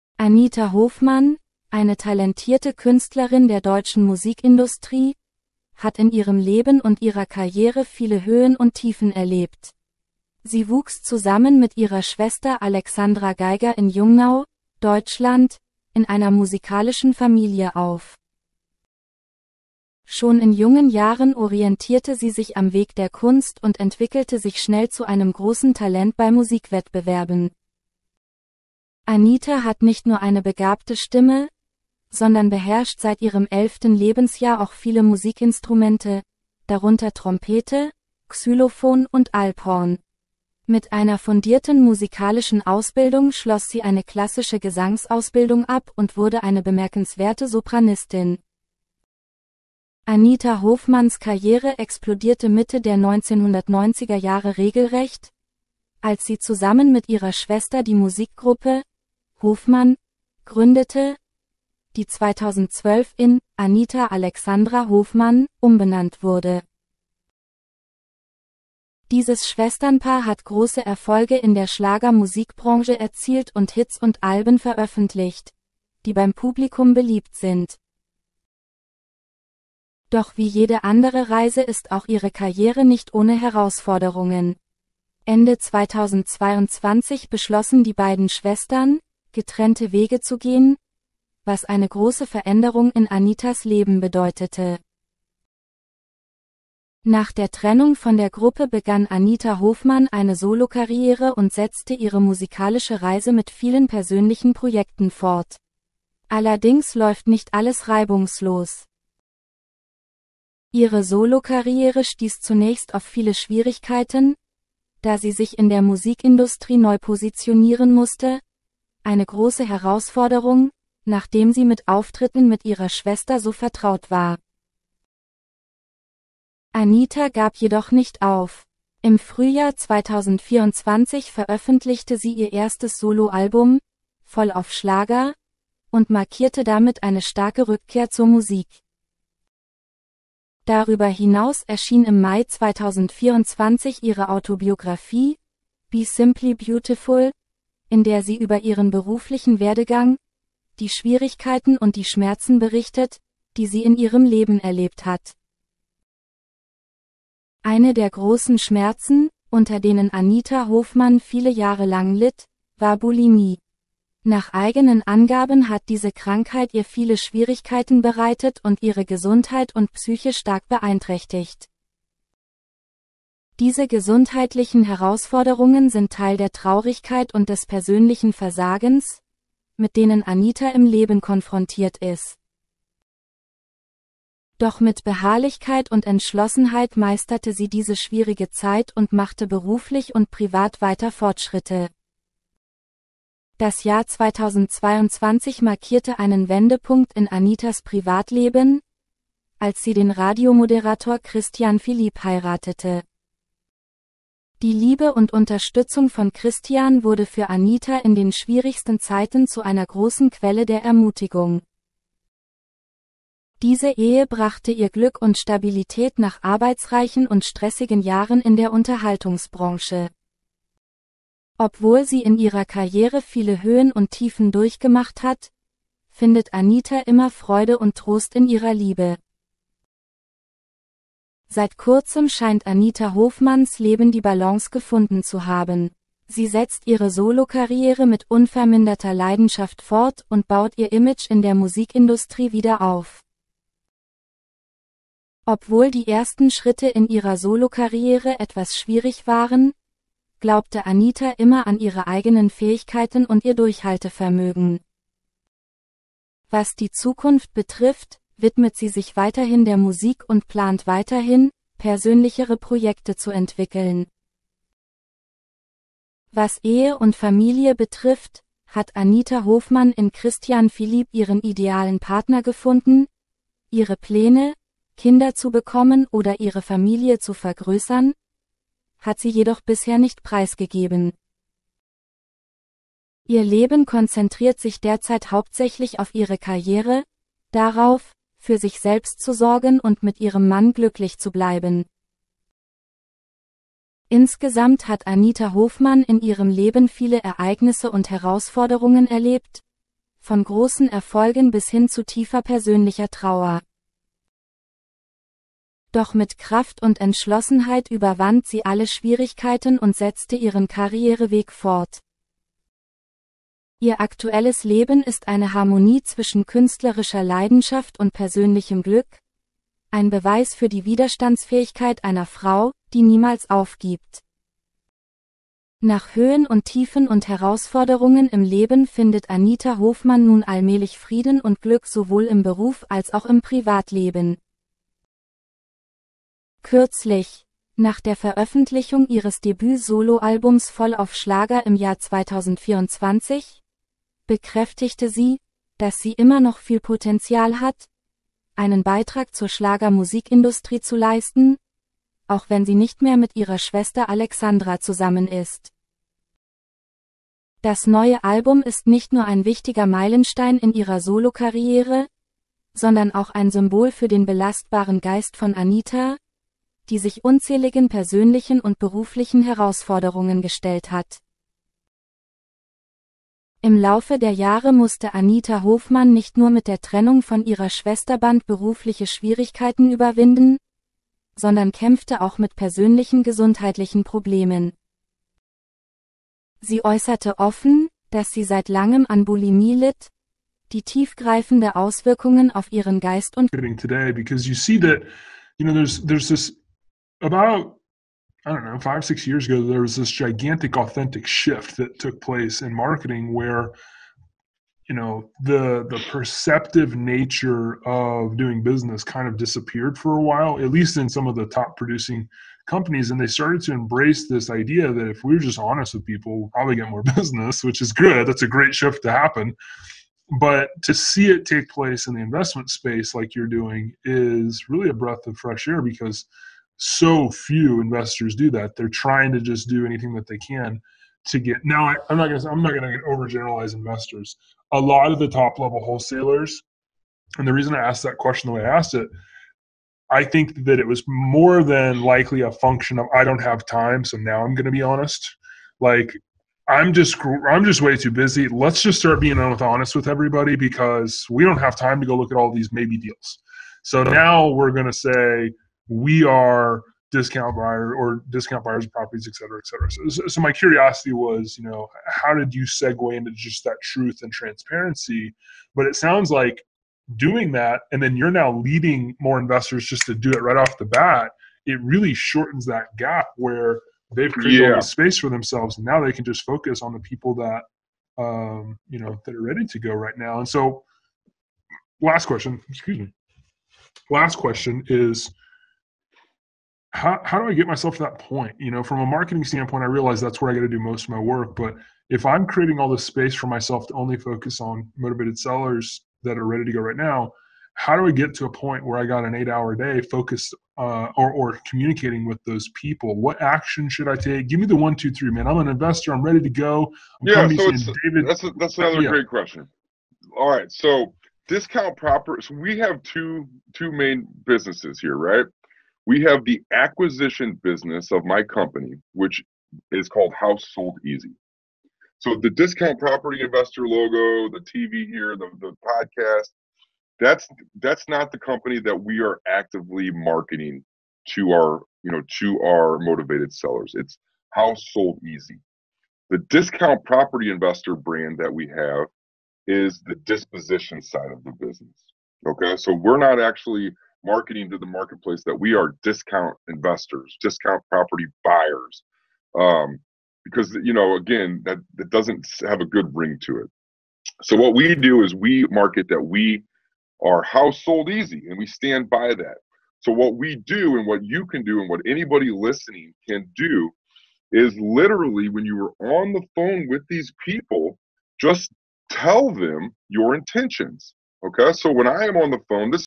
Große Trauer für Anita Hofmann – VOR 5 MINUTEN: Ihr Mann bestätigte die traurige Nachricht.
Anita Hofmann wuchs als Tochter von Josef Hofmann (1948–2023)[2][3] und Elisabeth Hofmann gemeinsam mit ihrer älteren Schwester Alexandra Geiger in Jungnau auf. Im Jahr 1988 zog sie mit ihrer Familie nach Meßkirch, wo sie bis heute lebt.
Bereits im Kindesalter stand Hofmann gemeinsam mit ihrer Schwester im Rahmen von Talentwettbewerben auf der Bühne. Sie erlernte ab ihrem elften Lebensjahr mehrere Instrumente, darunter Trompete, Xylophon und Alphorn.[4] Nach ihrem Schulabschluss absolvierte sie eine klassische Gesangsausbildung als Sopranistin.[5]